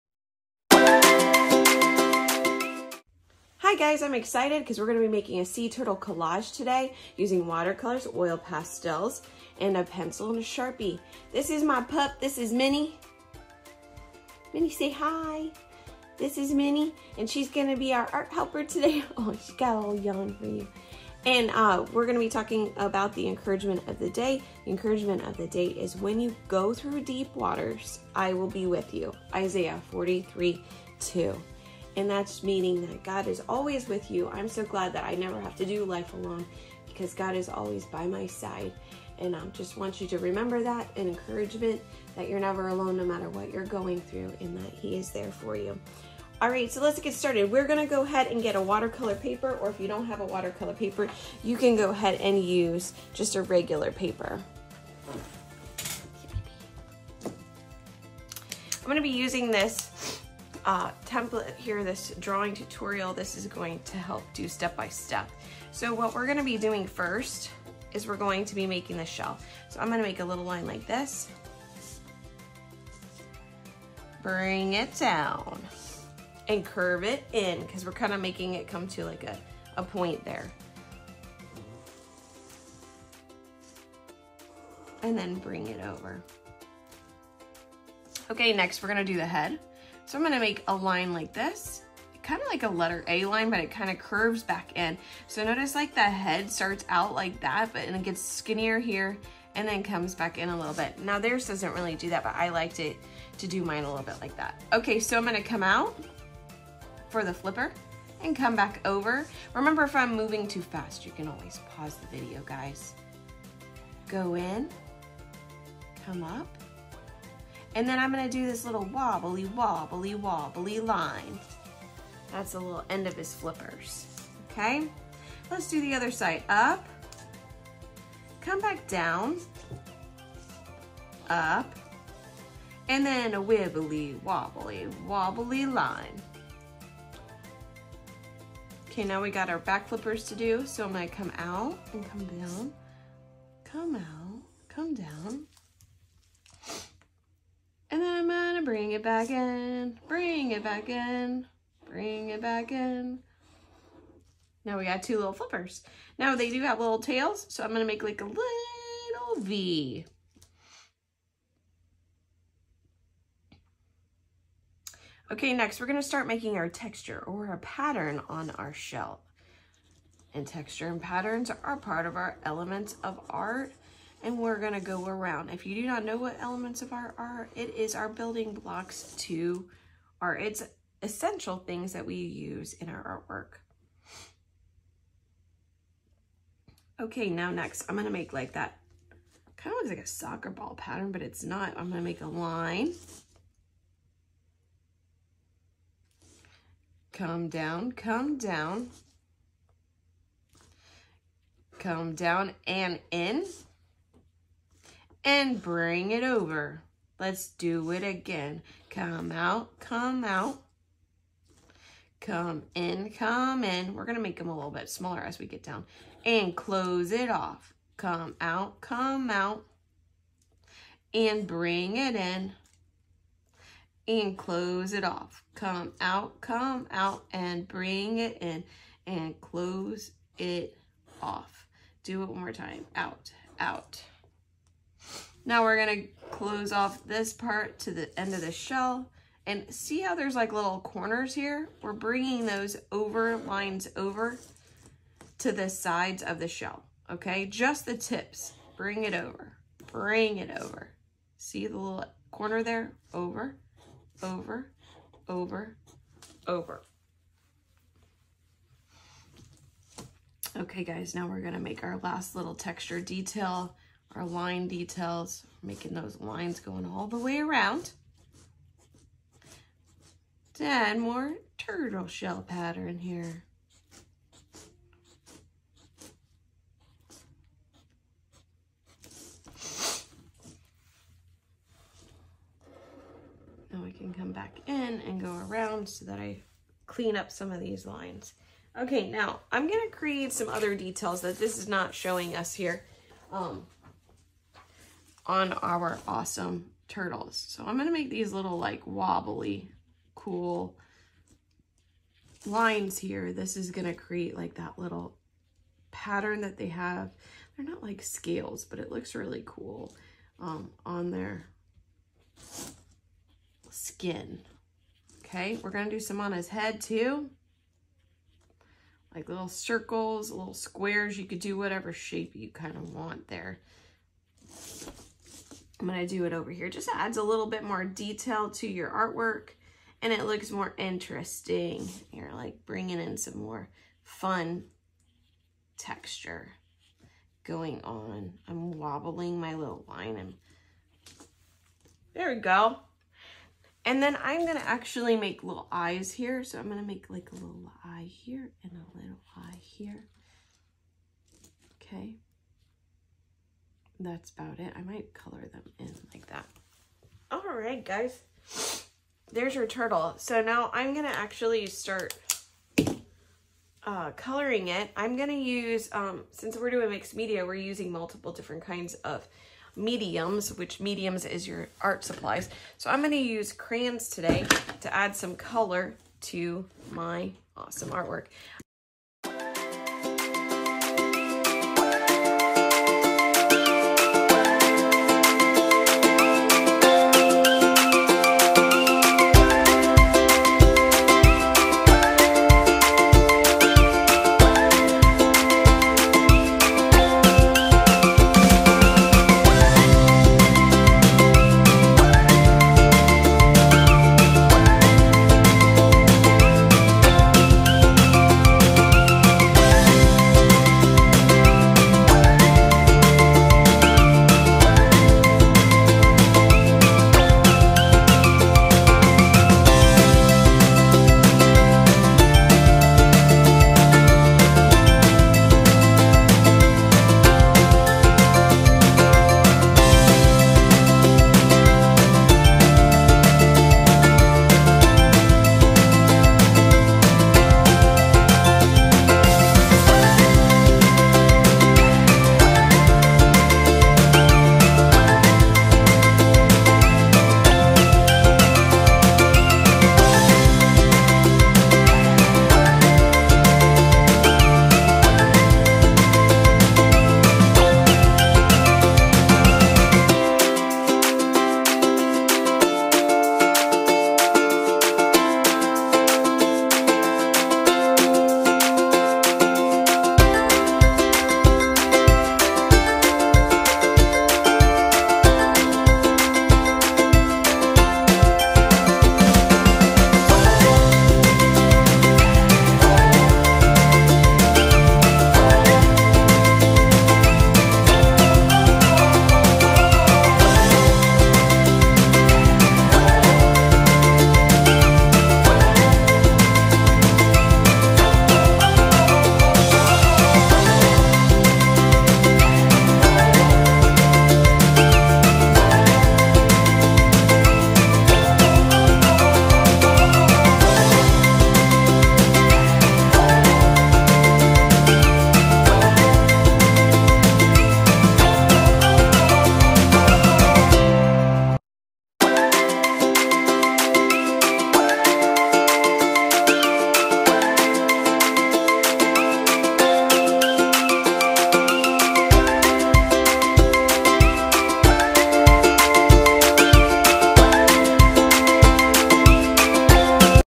Sea Turtle Collage
Learn how to make a sea turtle using watercolors.